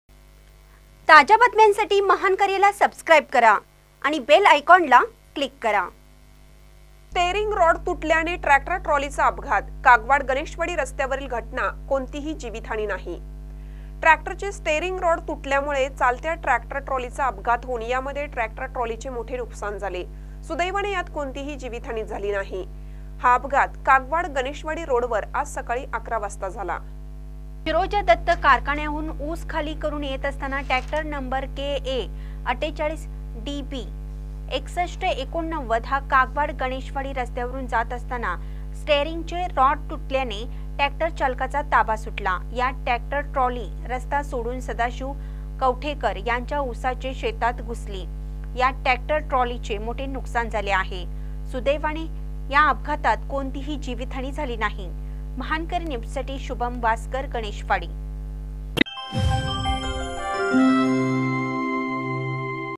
स्टेअरिंग रॉड तुटल्याने ट्रॅक्टर ट्रॉलीचा अपघात : कागवाड-गणेशवाडी रस्त्यावरील घटना
स्टेअरिंग रॉड तुटल्याने ट्रॅक्टर ट्रॉलीचा अपघात : कागवाड-गणेशवाडी रस्त्यावरील घटना, कोणतीही जिवितहानी नाही